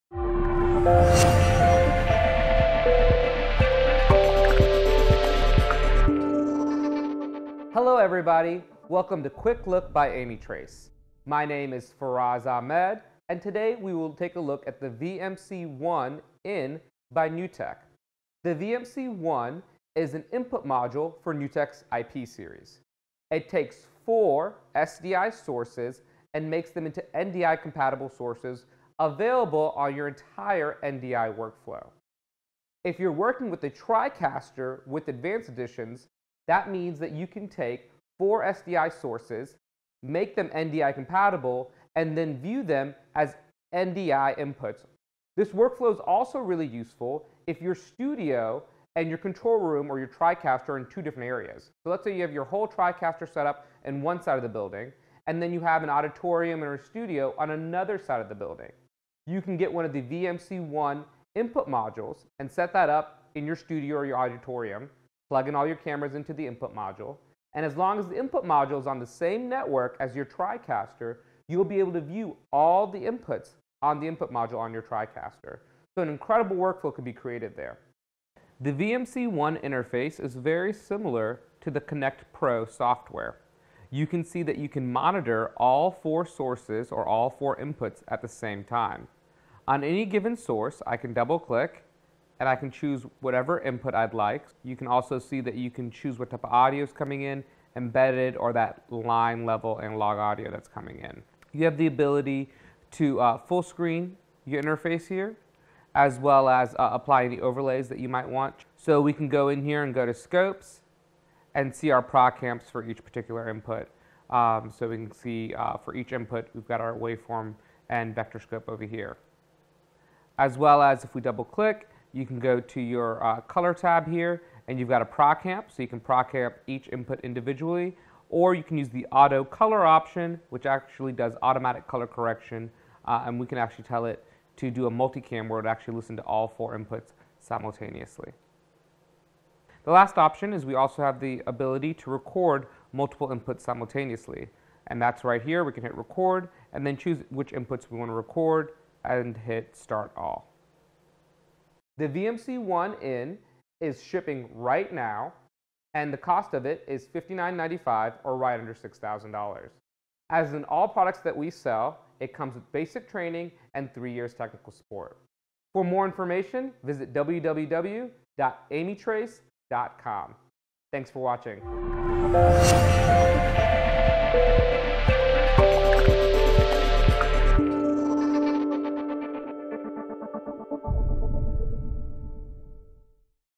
NewTek VMC1-In
Amitrace QuickLook at NewTek VMC1-In.  Expand the number of SDI inputs on your TriCaster with the VMC1-IN